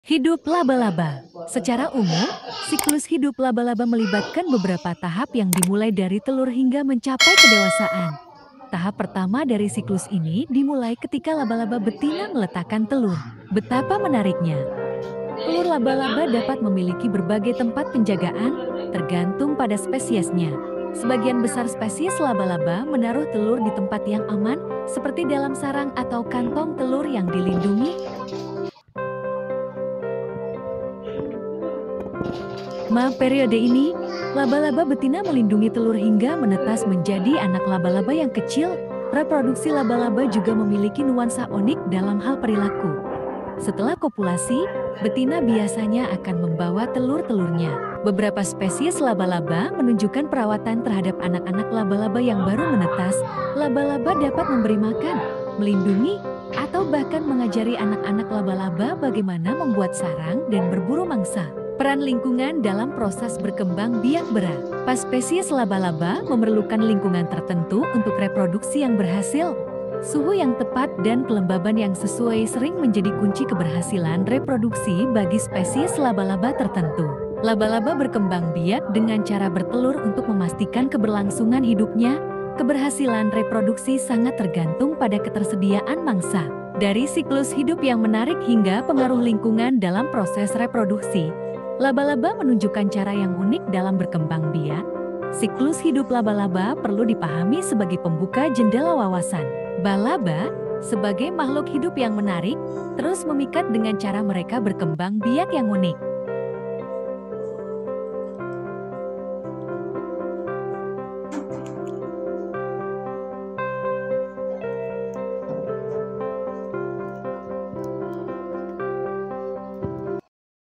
Siklus Hidup Laba Laba
Siklus hidup laba-laba terdiri dari tiga tahap, yaitu telur, anak laba-laba, dan dewasa. Berikut adalah penjelasan lebih lanjut mengenai siklus hidup laba-laba:

Telur

Laba-laba betina menyimpan sperma di wadah dekat ovarium, dan kemudian menggunakan sperma tersebut untuk membuahi telurnya. Laba-laba betina akan menyimpan telurnya di dalam kantung sutra yang dibuatnya khusus.

Anak laba-laba

Telur akan menetas menjadi anak laba-laba setelah beberapa minggu, tergantung pada kondisi lingkungan. Anak laba-laba akan menyerupai laba-laba dewasa mini, dengan delapan kaki, satu kepala, dan satu tubuh.

Dewasa

Anak laba-laba akan tumbuh menjadi dewasa melalui proses pergantian kulit. Laba-laba betina dewasa akan bertelur lagi, dan siklus hidup laba-laba akan dimulai lagi.

Laba-laba menjalani proses yang disebut "metamorfosis tidak sempurna". Proses ihni berbeda dengan metamorfosis sempurna yang dialami serangga, seperti kupu-kupu dan kumbang.

Rentang hidup laba-laba bervariasi, ada yang kurang dari satu tahun dan ada yang bisa hidup hingga dua puluh tahun.